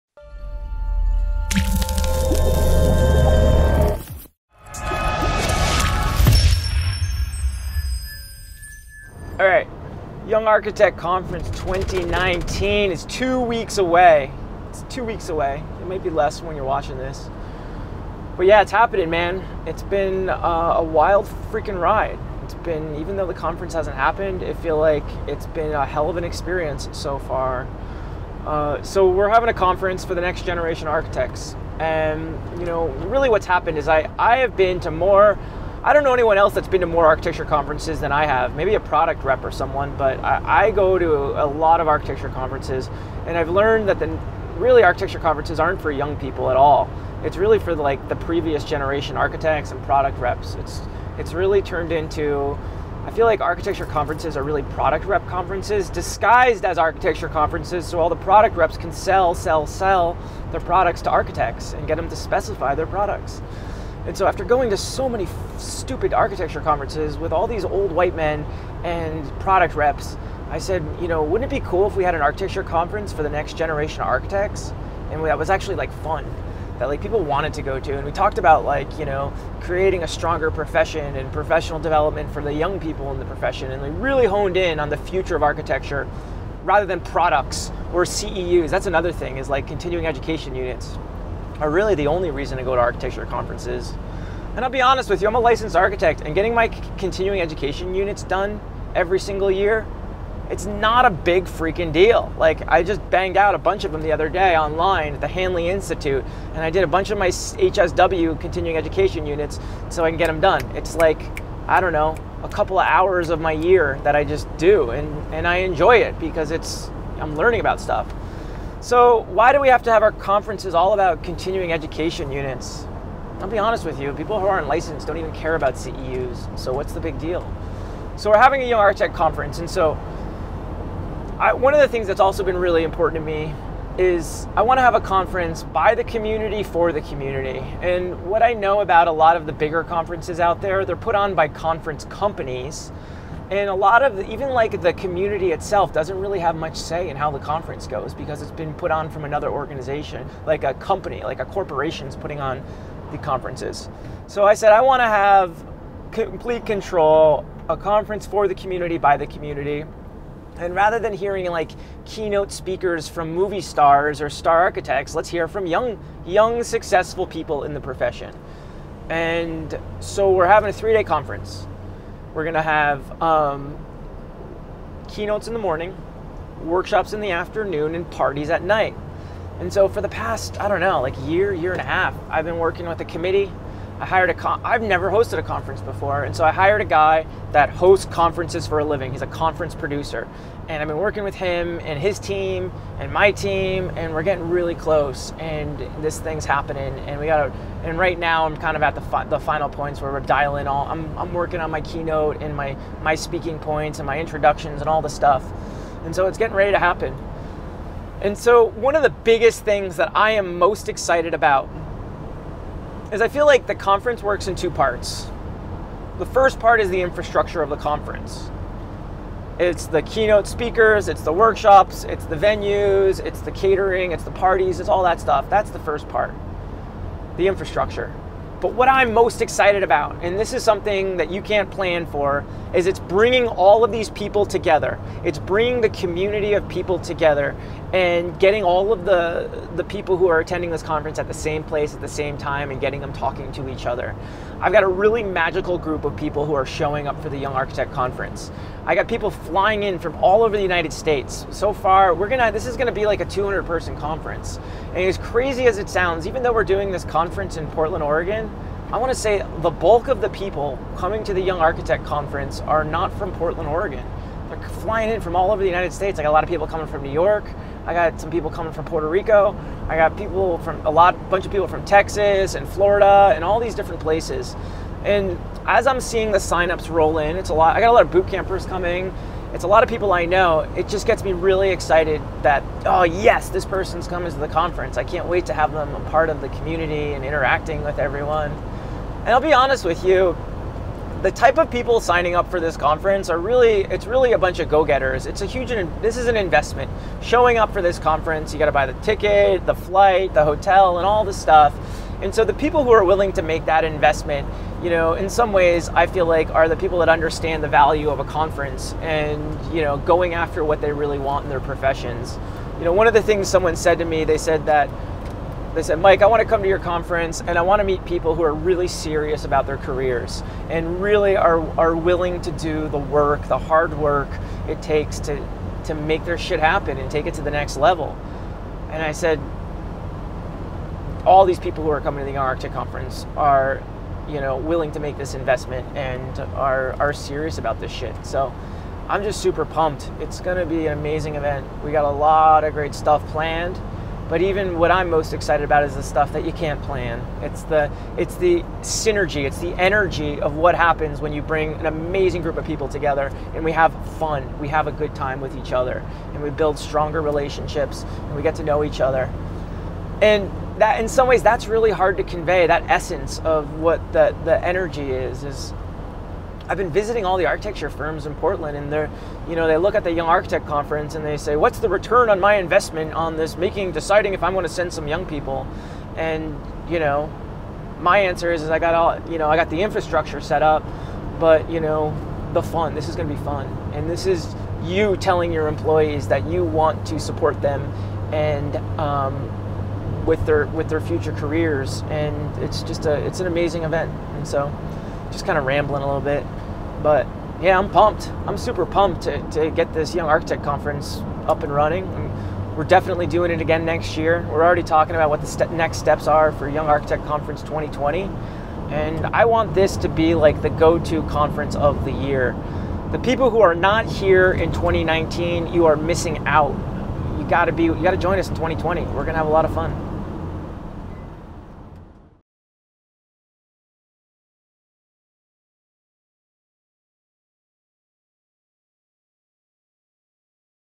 MIKE IN VAN TALKING
Michael Riscica AIA Discussing the Young Architect Conference while  driving to the Oregon Coast.